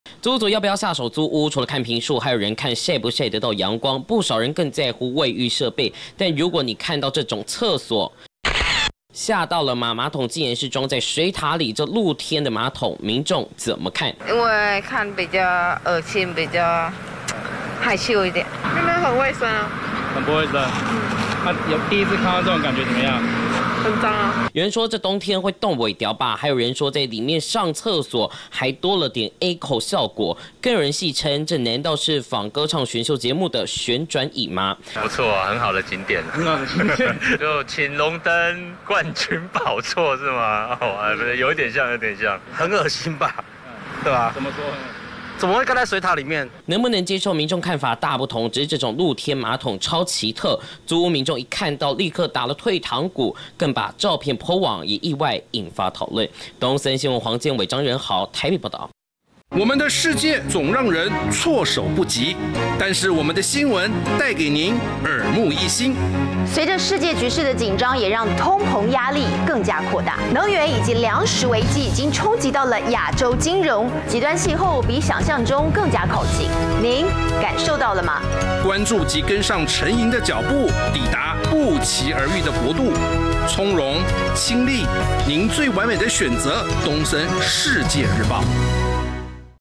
詭！租屋驚見奇葩馬桶 網：早期五燈獎寶座「走鐘版」@ebcrealestate
馬通裝在水塔裡！這樣的衛浴設備，你會願意租嗎？雖然很通風，但沒有門的設計，讓租屋客立刻打退堂鼓。不過這畫面倒是很多網友說，聯想到早期人氣節目「五燈獎」的衛冕者寶座。